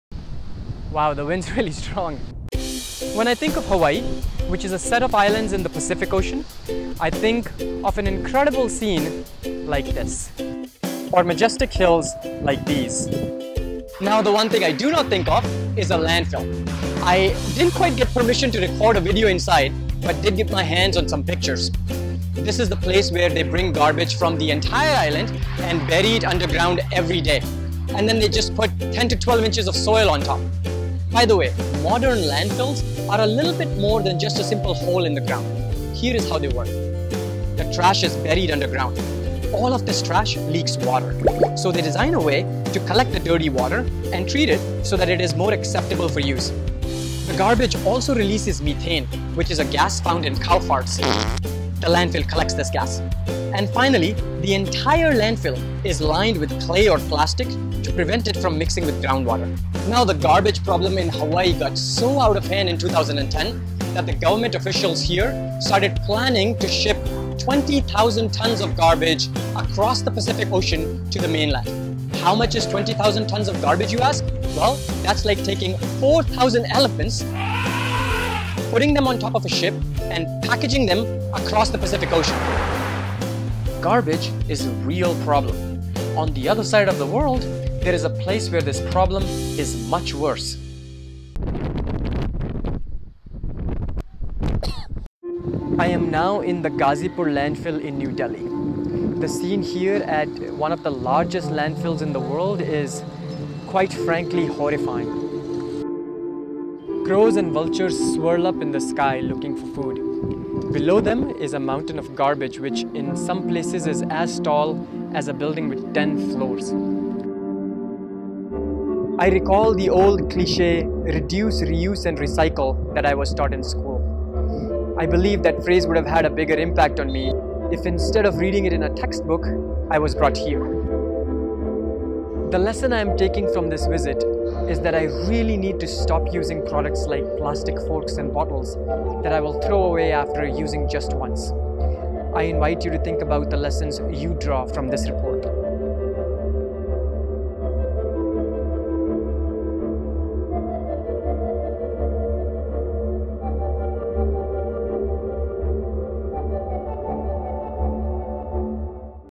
How Landfills Work [Class 6, Chapter 16, Garbage In Garbage Out, NCERT]
This video explores interesting ideas from Chapter 16, Garbage In Garbage Out, in the 6th grade science textbook.

We take a journey through landfills in Hawaii and New Delhi.